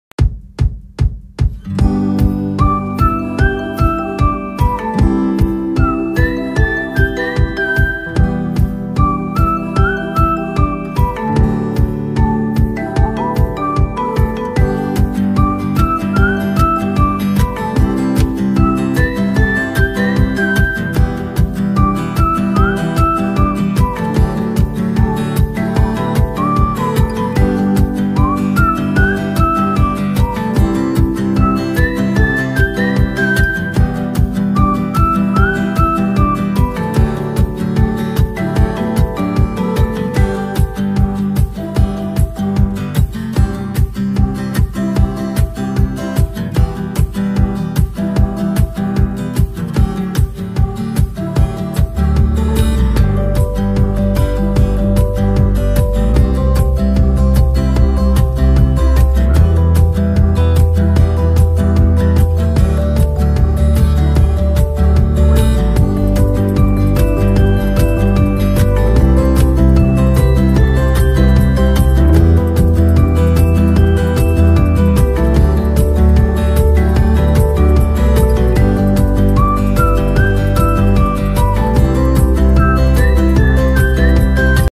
Mavic Air 2 over Luxury Beach Resort, Ghana 🇬🇭 West Africa
A nice beach to visit. Mavic Air 2 shot in 4K